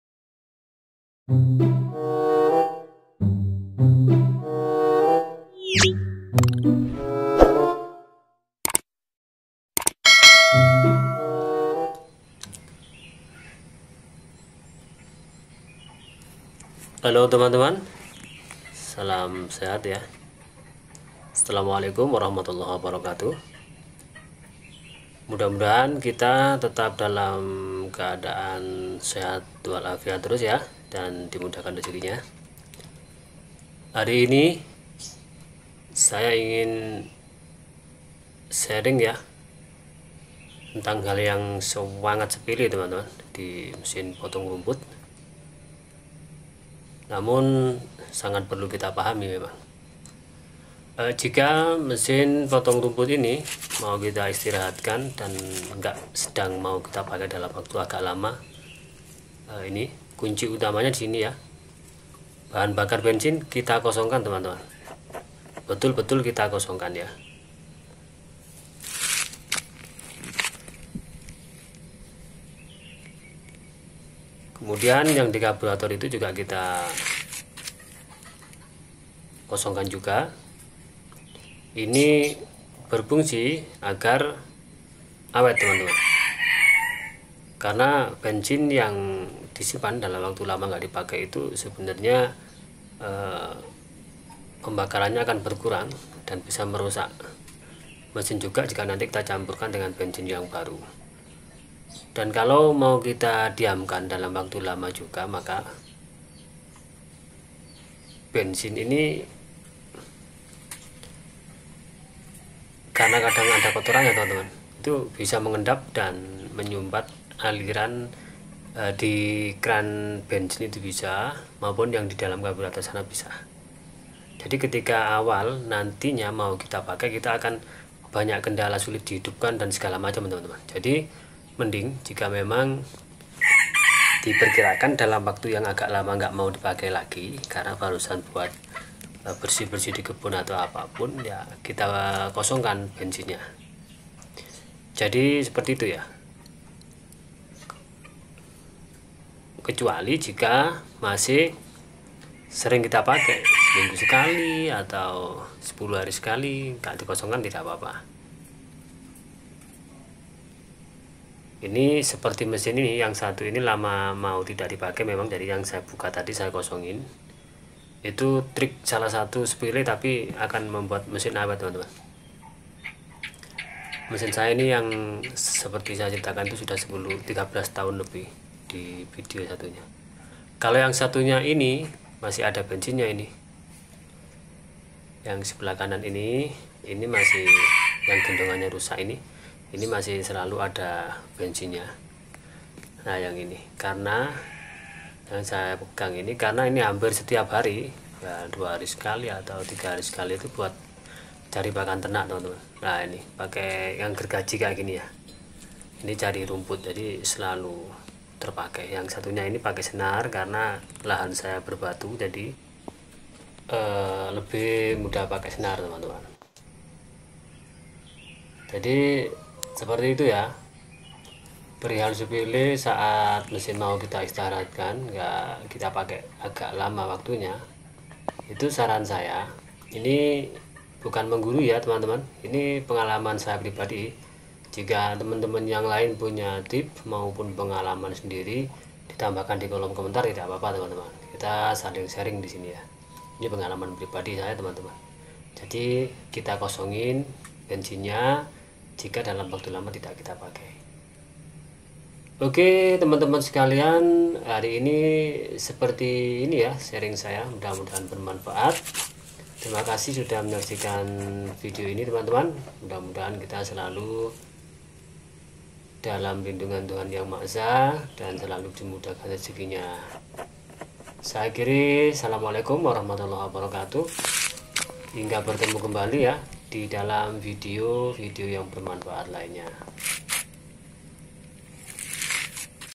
mesin potong rumput jarang dipakai ,ini yang harus dilakukan agar awet tidak mudah rusak
hal sepele yang harus dilakukan agar mesin rumput tdk mudah rusak padahal jarang dipakai